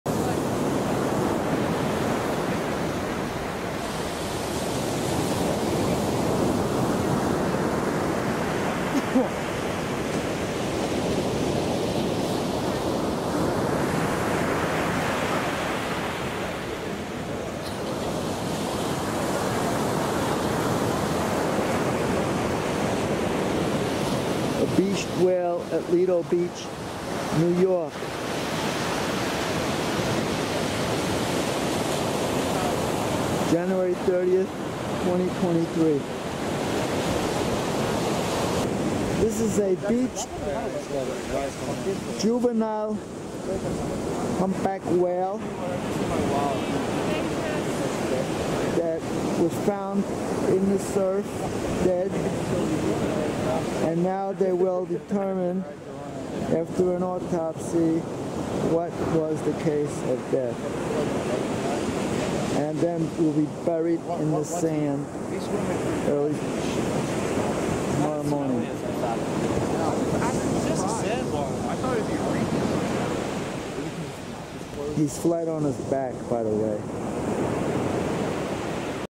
A beached Humpback whale at Lido Beach NY
A dead juvenile Humpback whale was found at Lido Beach this morning.
 An autopsy will determine the cause of the whale's death.